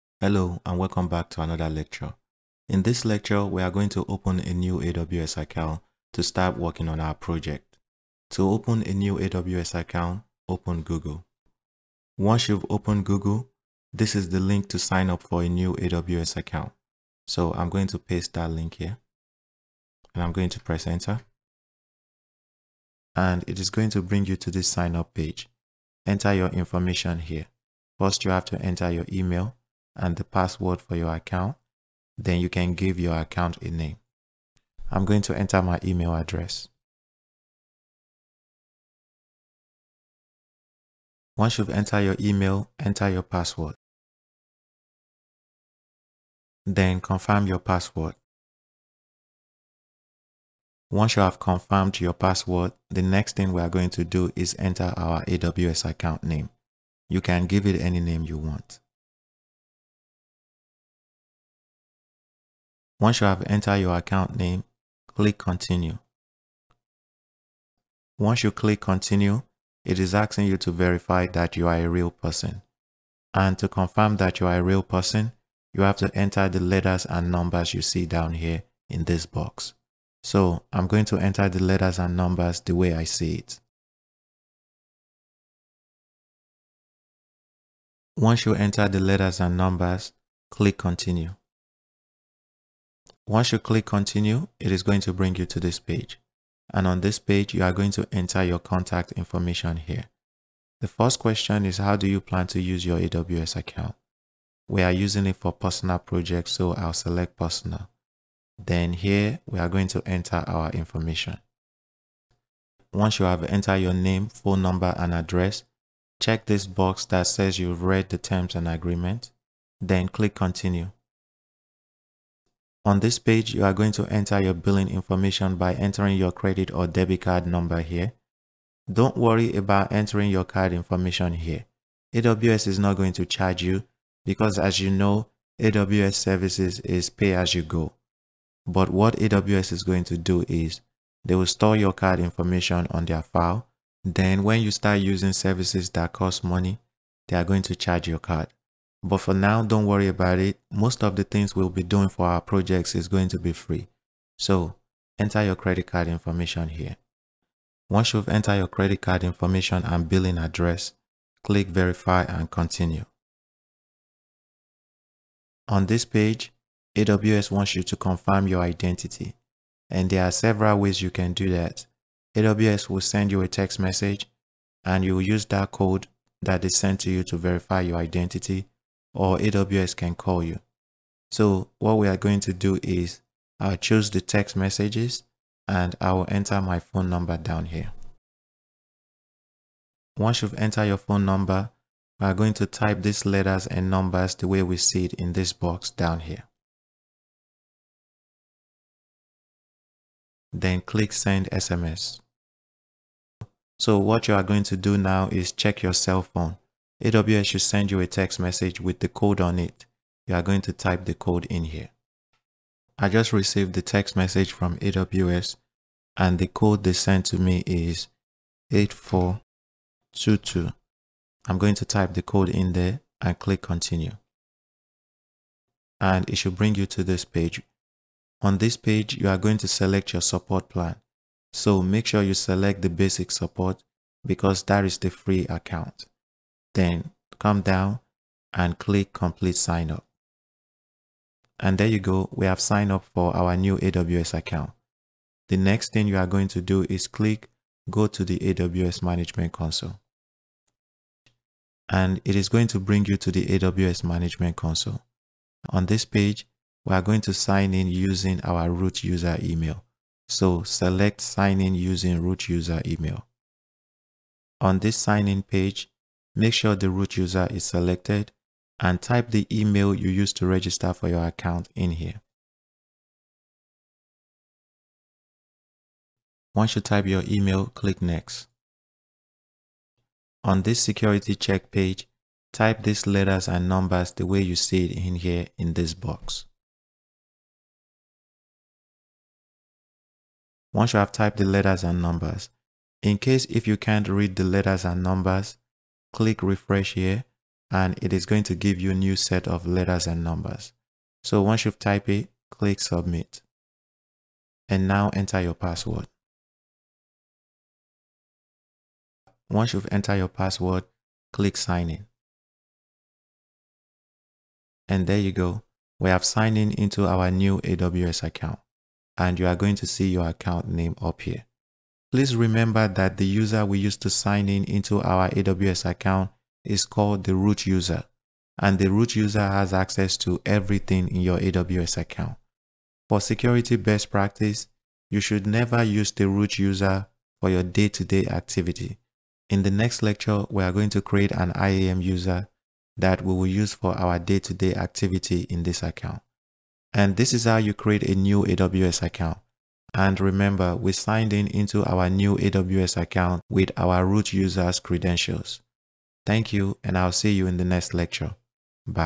How to Sign Up for a New AWS Account
In this video, you will learn how to open a new AWS account and sign in as a root user.

AWS Hands-on Projects for Beginners👇
Projects Coming Soon 👇
✔ Deploy a Website with Terraform Modules, AWS ECR, and AWS ECS

- Topics Covered -
0:00 Intro
0:07 How to sign up for a new AWS account
4:05 Sign in as the root user